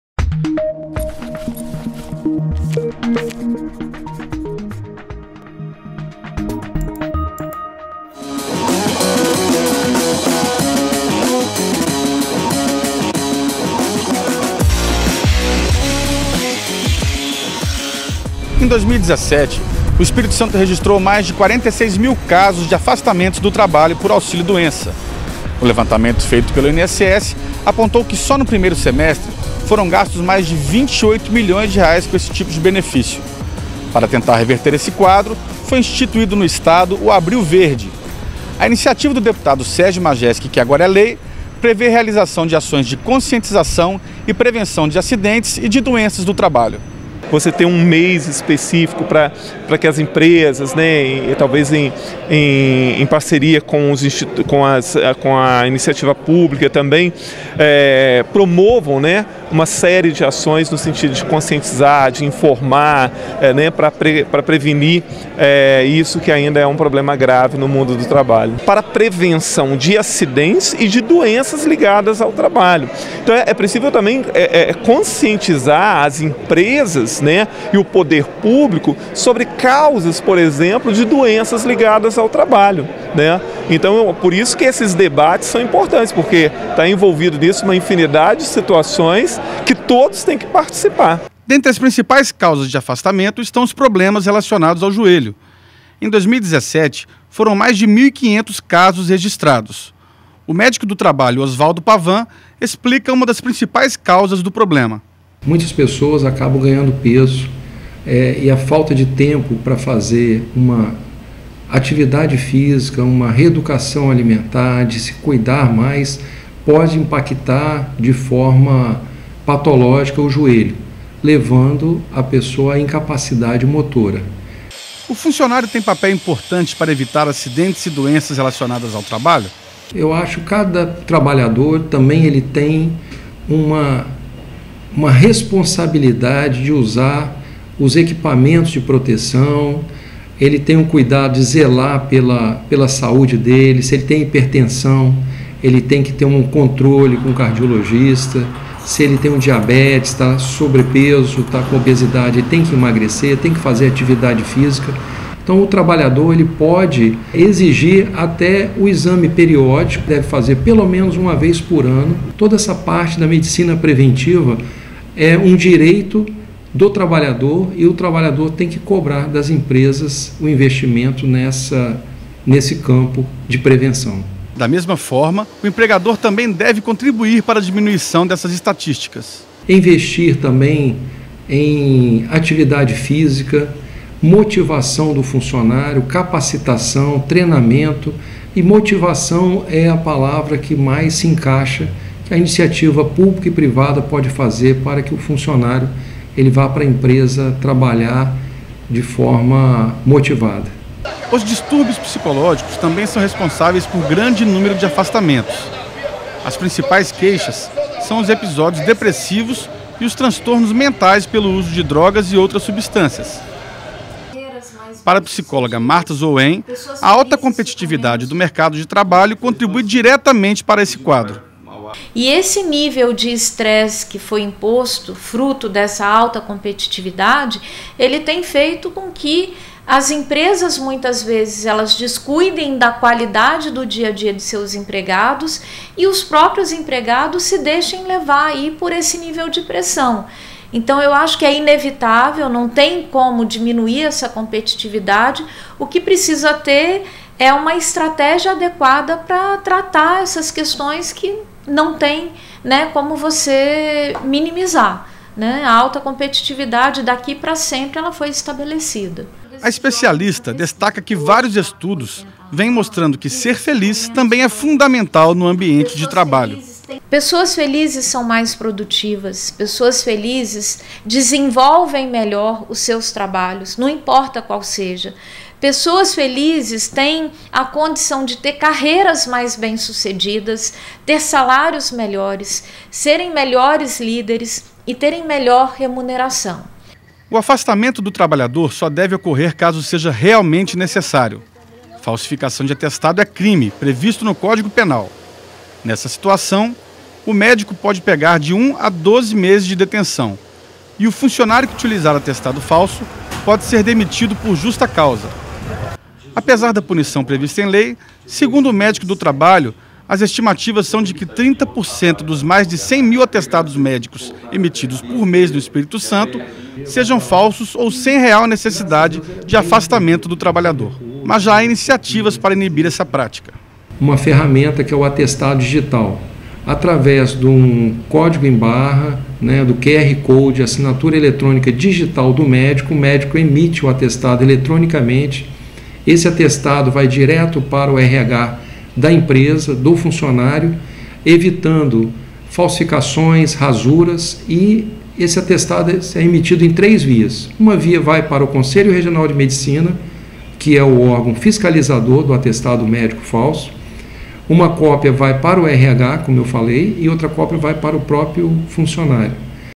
Prevenção de Doenças e Acidentes de Trabalho (Abril Verde) - Agora é Lei
Lei aprovada pela Assembleia Legislativa institui o "Abril Verde", mês dedicado a campanhas visando a prevenção de doenças e acidentes de trabalho.